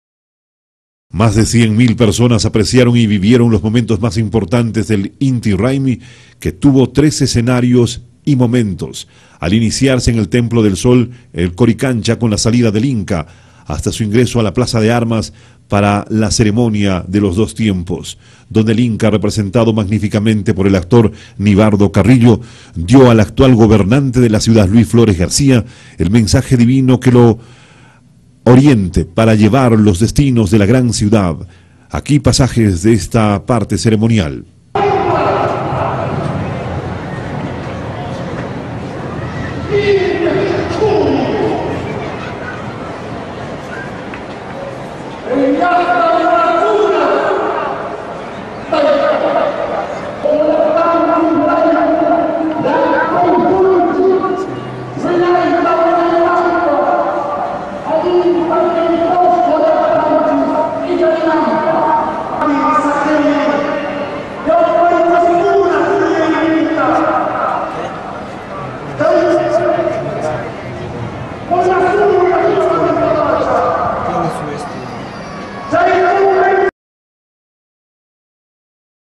Inti Raymi 2012: más de 100 mil turistas en Cusco
CUSCO.- El cusco festejó la celebración del Inti Raymi como actividad central por el aniversario de la ciudad. Más de 100 mil turistas entre peruanos y extranjeros gozaron del espectáculo. Veamos el informe.